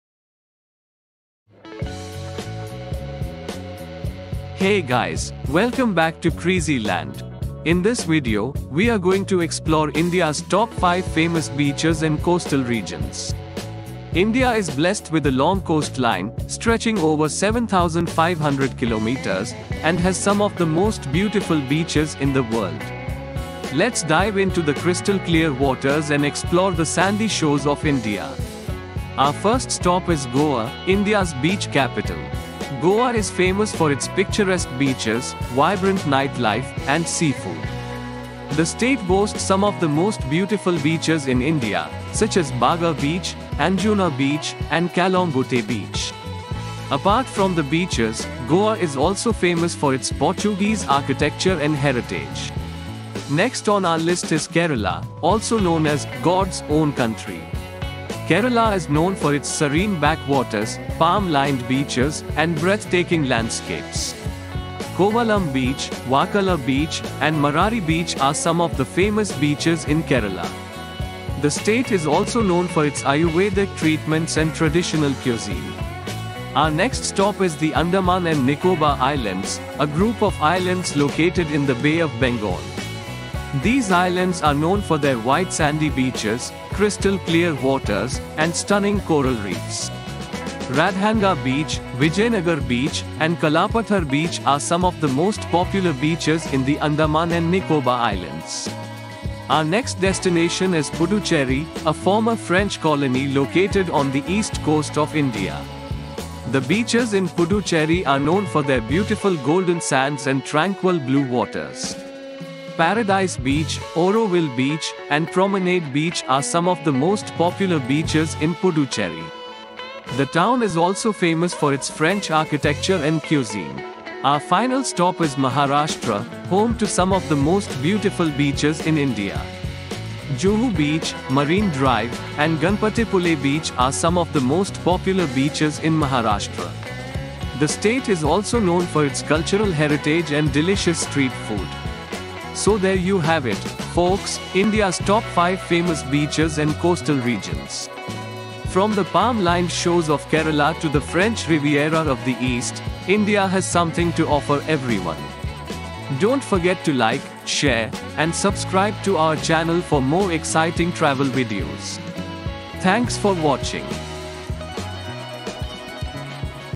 Top 5, India's famous beaches and coastal regions
In this video, we explore India's top 5 famous beaches and coastal regions. From the vibrant nightlife of Goa to the tranquil backwaters of Kerala, India has something to offer everyone. Join us on this adventure as we dive into the crystal clear waters and explore the sandy shores of India.